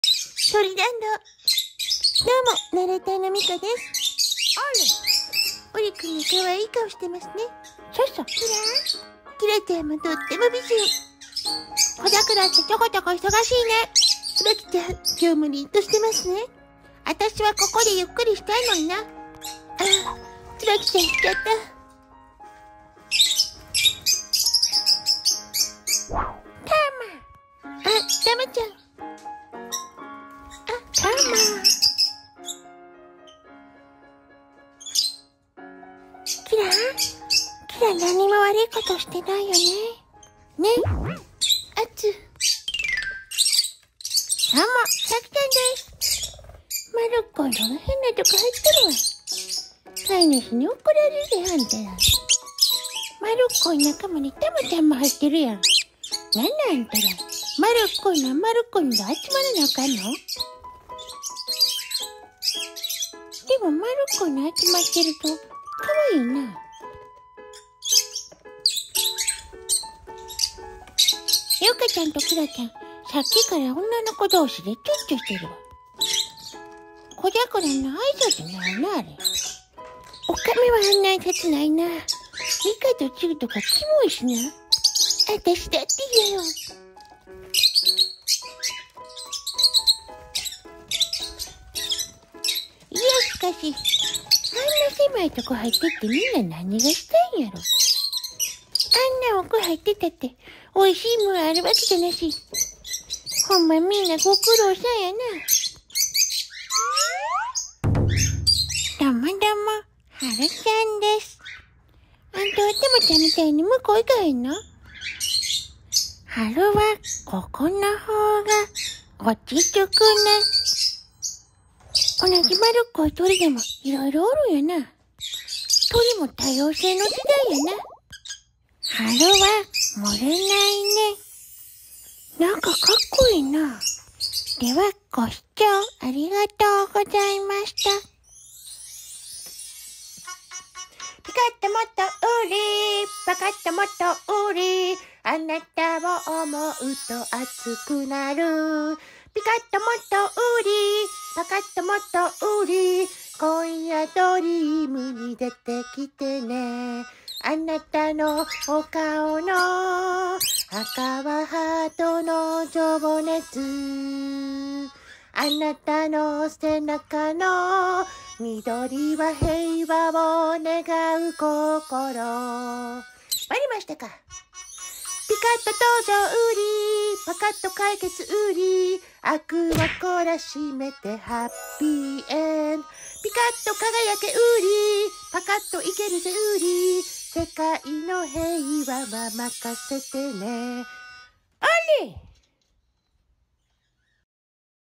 コザクラ＆ボタン＆桔梗普通にごちゃ混ぜで遊ぶ！多頭飼い朝☀️の一斉放鳥🕊️🦜🐤ナレーター未迦と関西系サキ
うちの朝の様子です